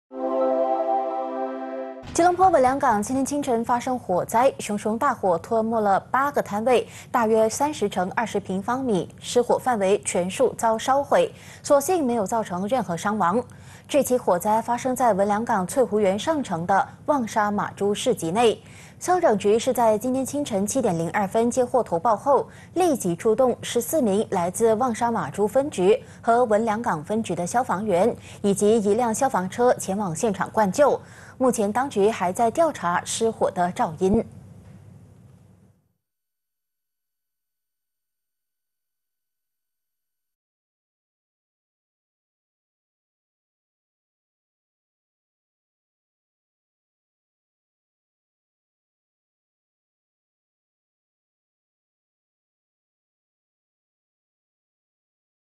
隆市文良港今晨火魔光顾 旺莎玛珠市集8摊位全毁【2024.07.31 八度早新闻】
【隆市文良港今晨火魔光顾  旺莎玛珠市集8摊位全毁】
文良港翠湖园上城(Danau Kota Uptown)今早发生火灾，8个摊位付之一炬。

【今日主播】周秀洋